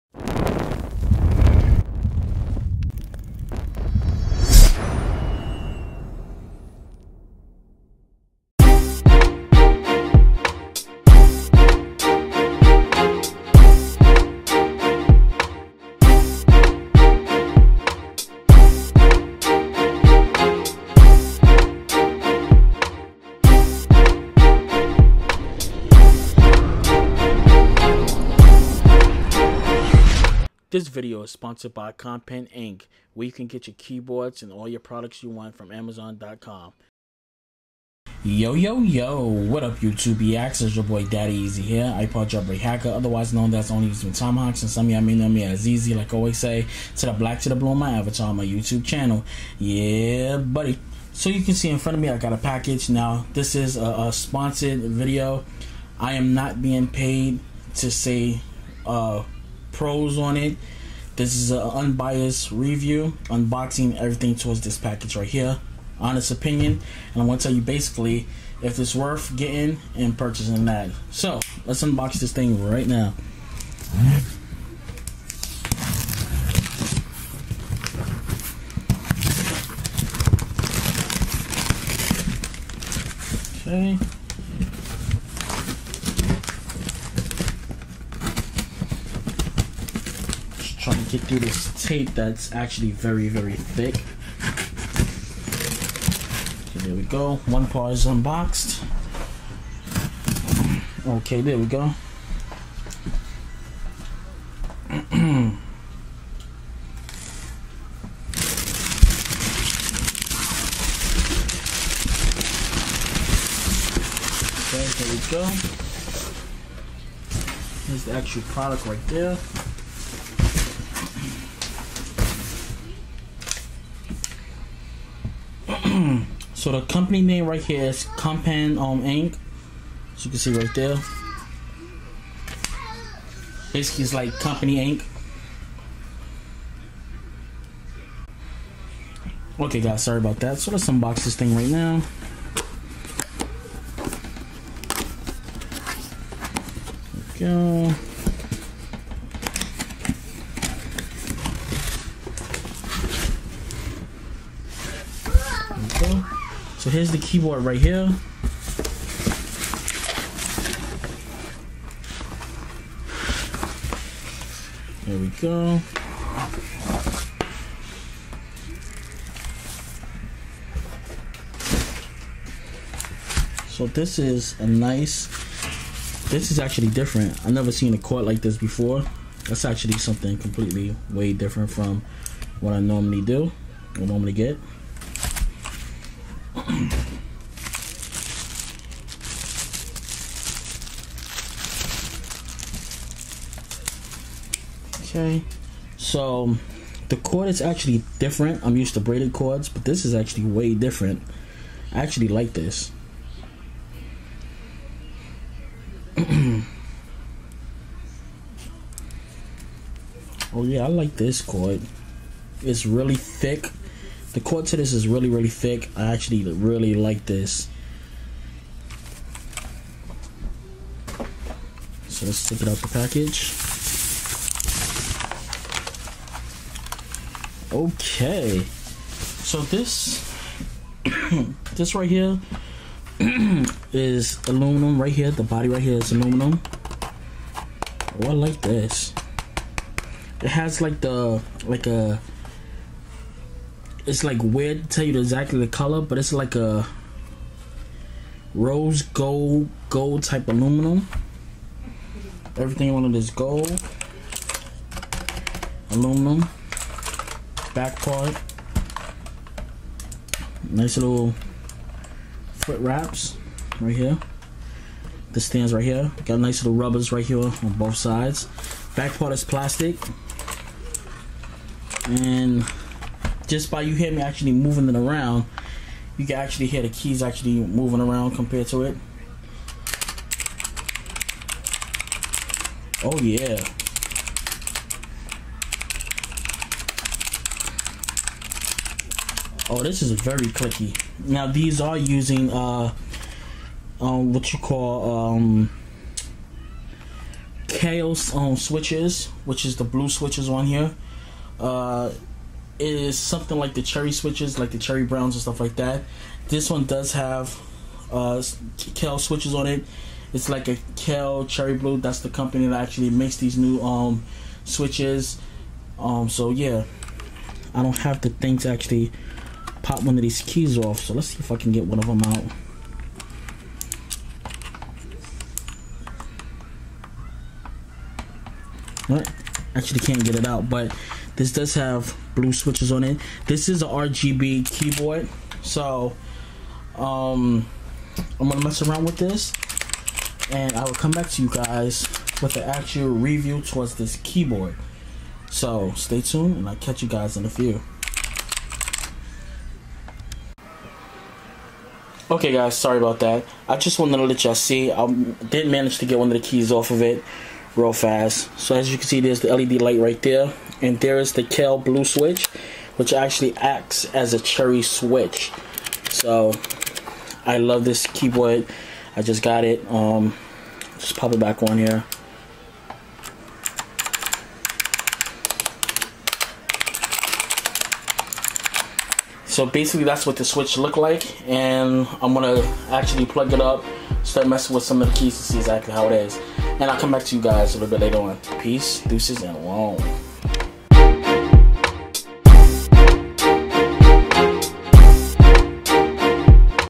Unboxing Ousaid Aluminum Clicky Colorful RGB BL Gaming Mechanical Keyboard With KAILH Blue Switches
To Join The Curse Network Union For Gamers Hit The Link Below Use This Link to Join

Twitch.tv/daddyezee